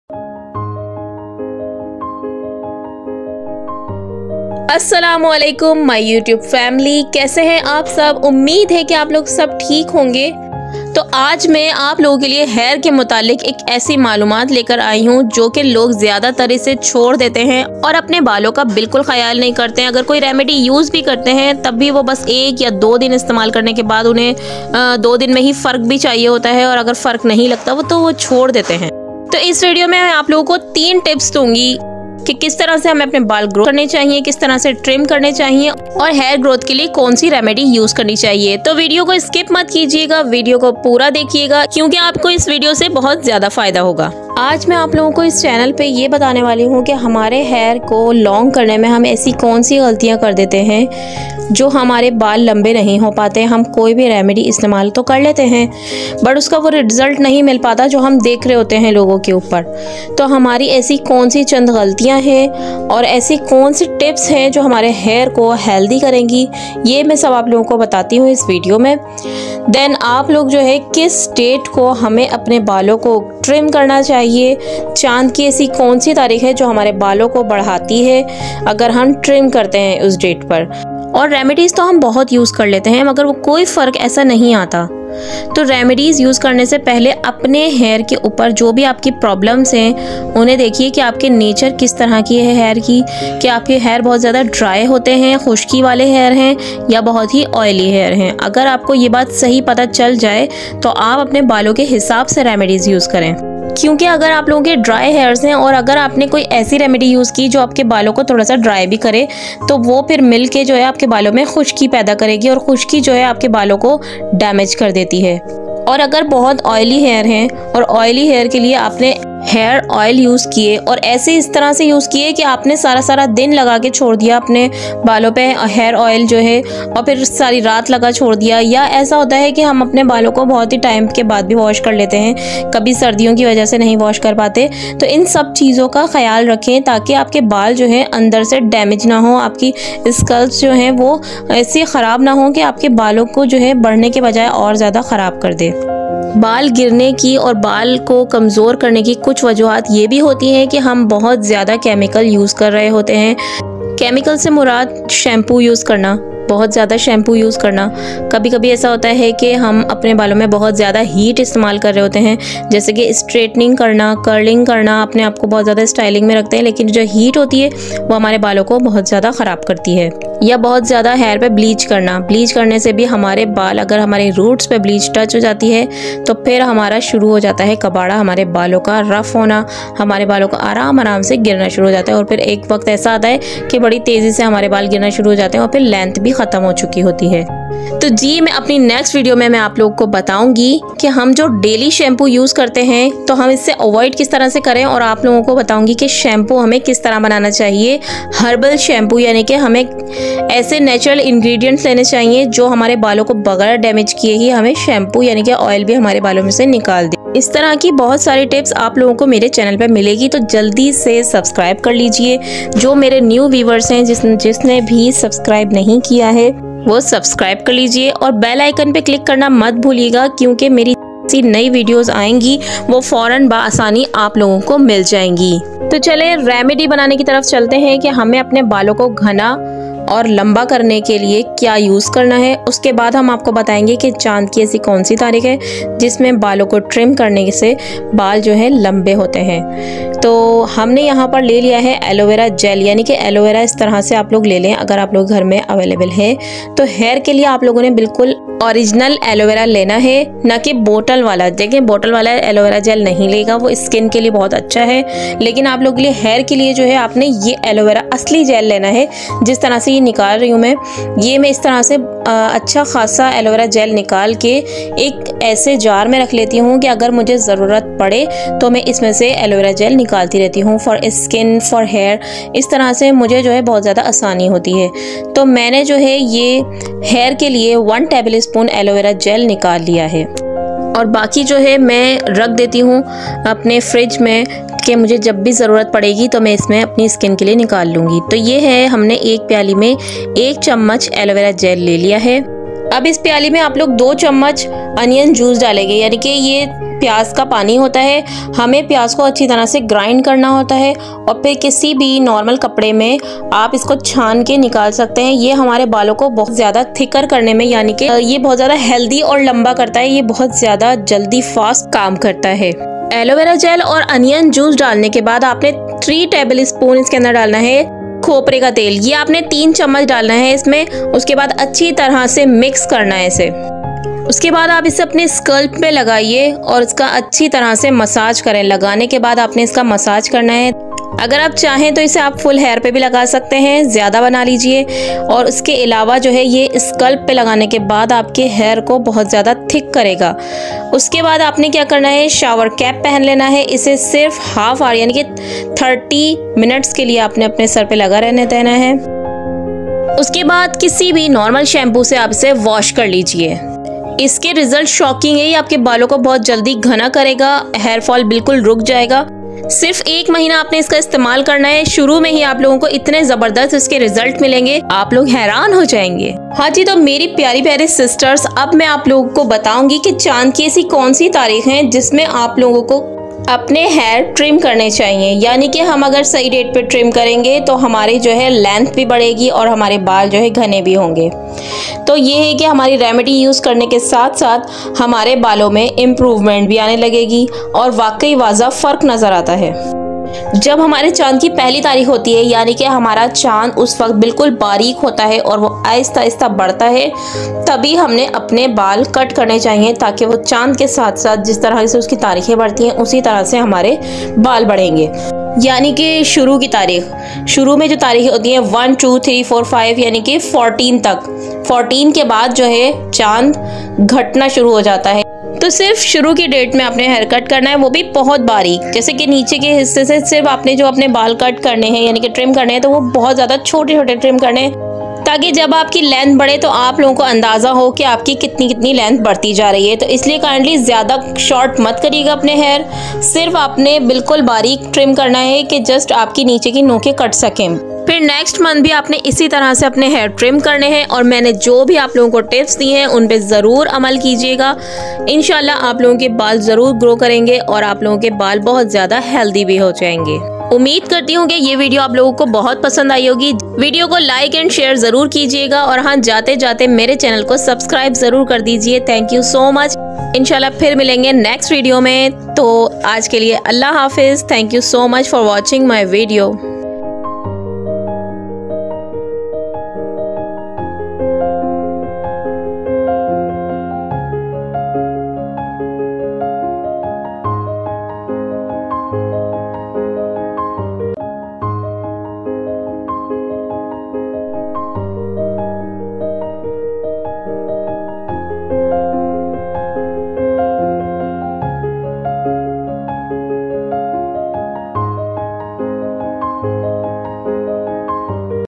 Hair growth remedy and important tips and secrets for fast growth || hair trim according to moon
hey Meri YouTube family Assalamualaikum

is ak video me mene wo tamam tareeke btaen hen jis se hamare hair long or healthy honge . ☺️☺️☺️
or how to trim our hair according to moon..🌚🌚🌛..